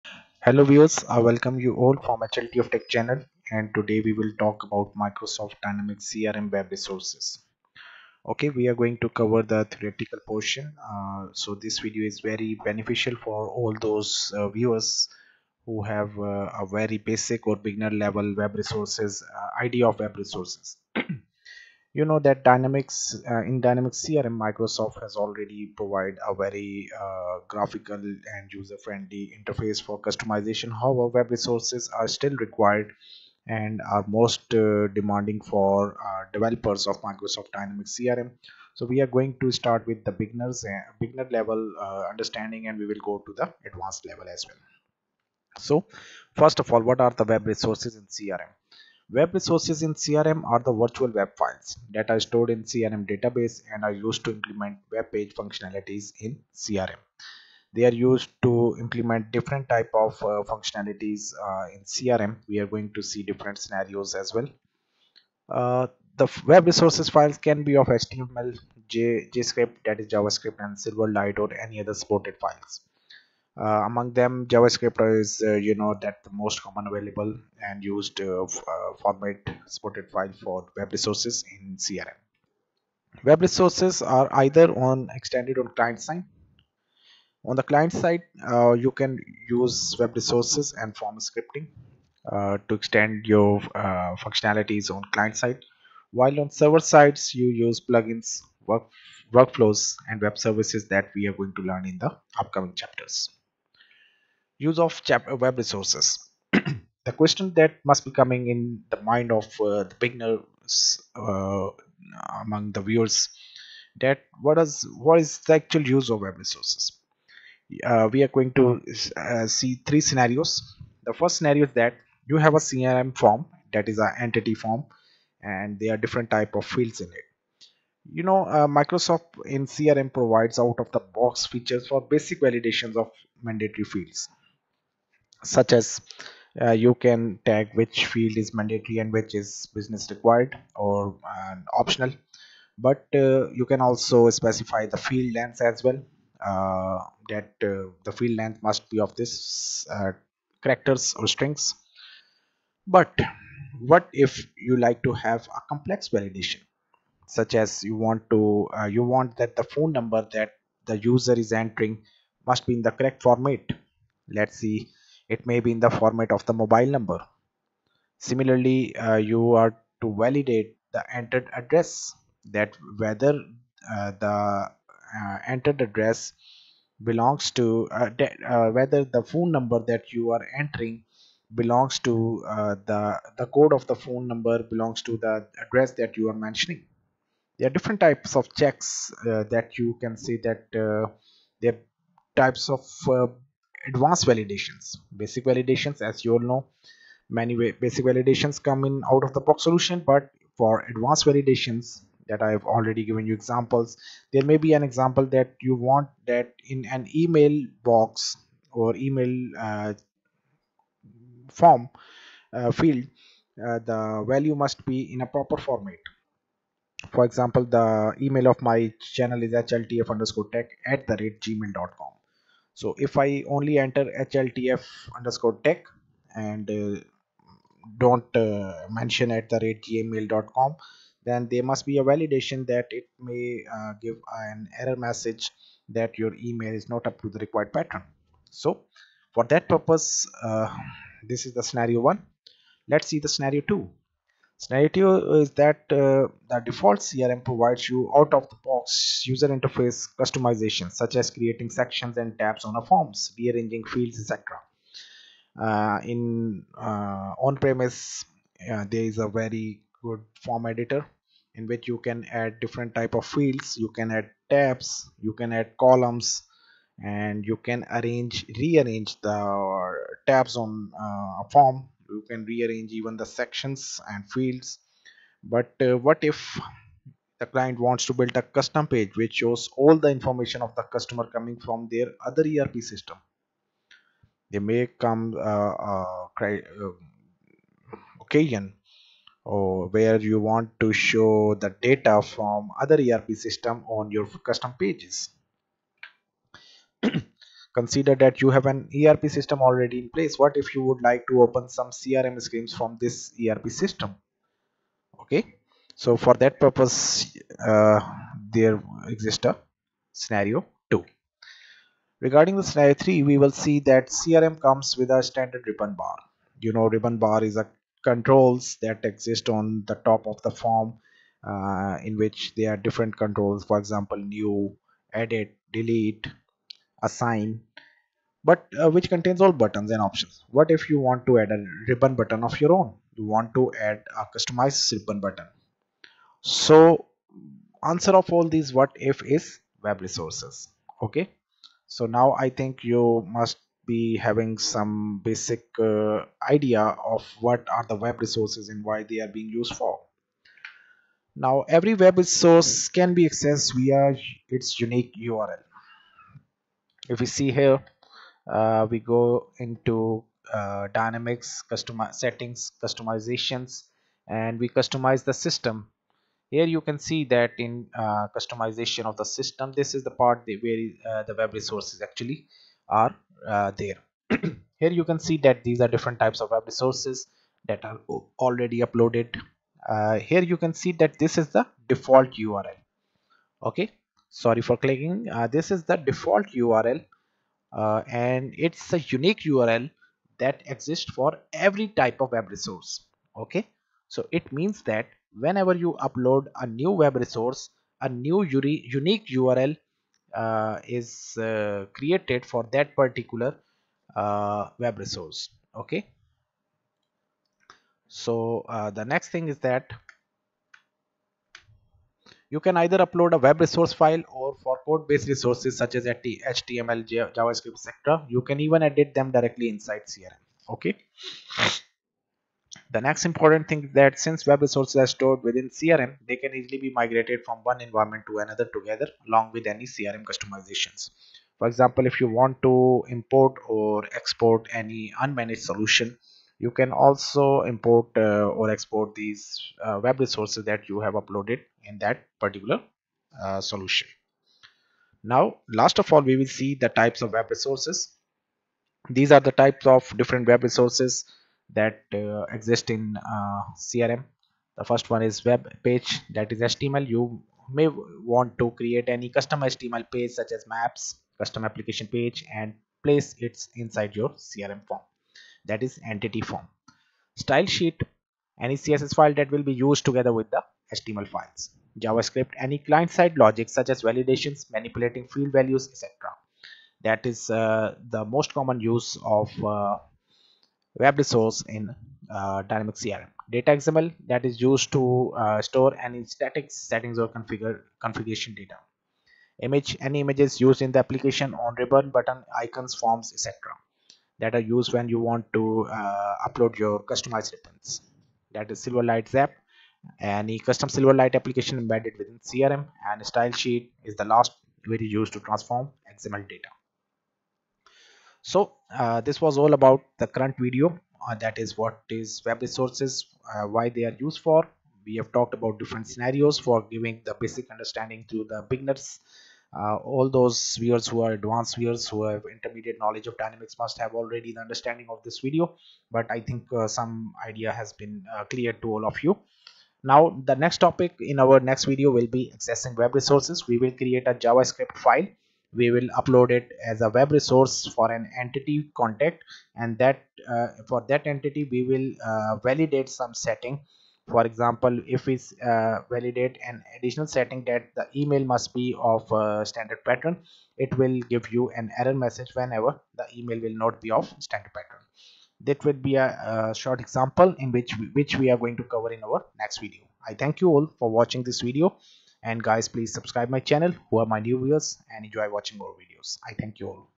Web Resources in Dynamics CRM explained | Form Scripting using WebResources
Web resources in dynamics crm are very useful for forms customization also known as form scripting.

This video is a theoretical lecture on undertanding Web Resources in Dynamics CRM. Web resources are files of html, Javascript, silverlight, css, xml, images such as png, jpg. gif, ico l, silverlight (xap) and style sheet etc and are used for forms scripting. It is used for applying validations of entity forms attributes, calling external web services in your server side plugin code etc.